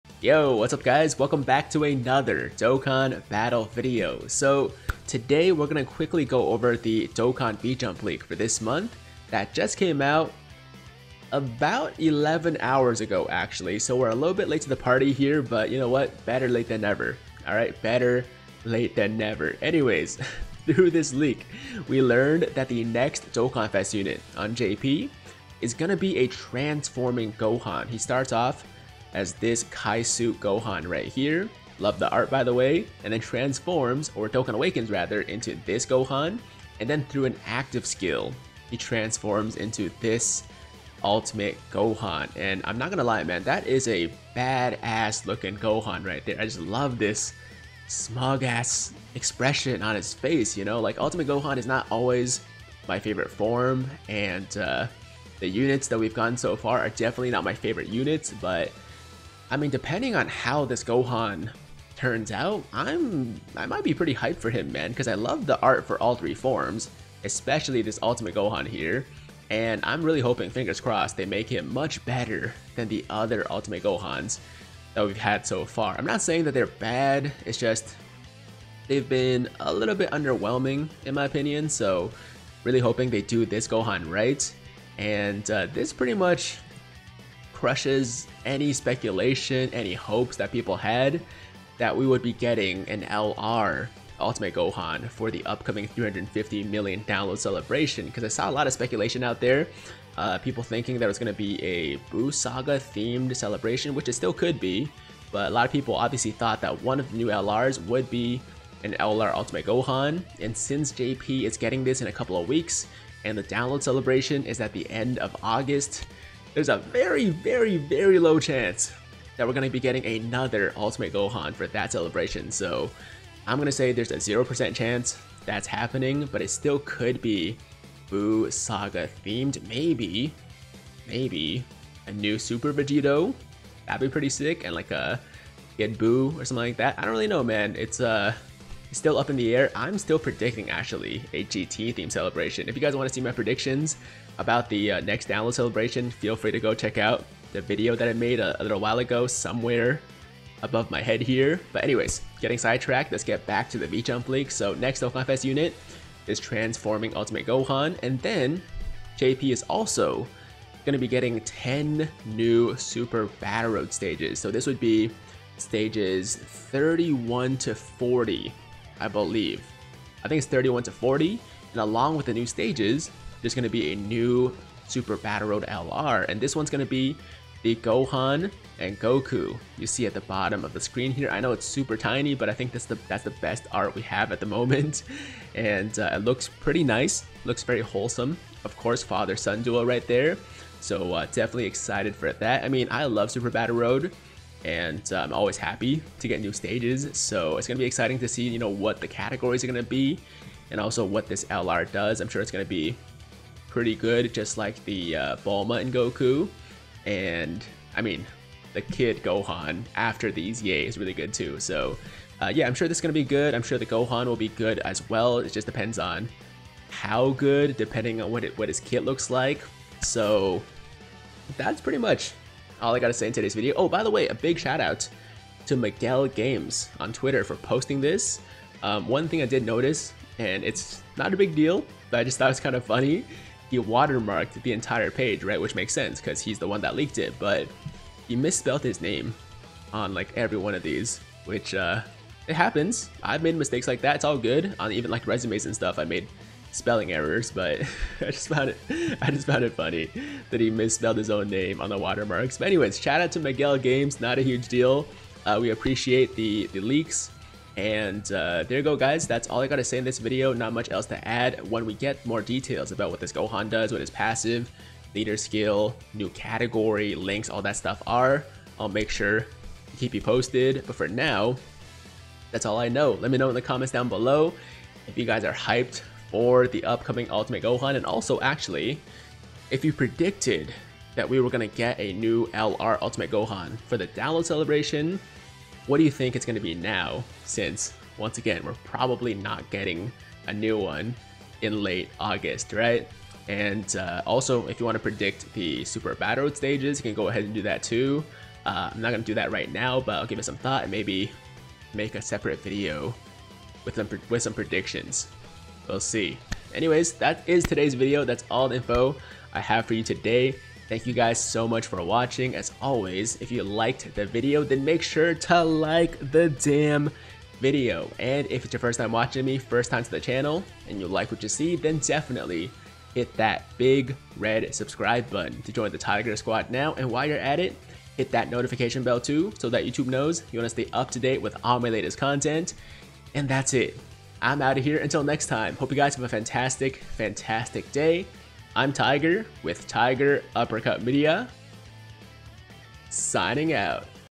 NEW SBR STAGES & LR + TRANSFORMING ULTIMATE GOHAN!! V-Jump [July 2020] | Dragon Ball Z Dokkan Battle
Here's everything we learned from the July 2020 issue of V-Jump!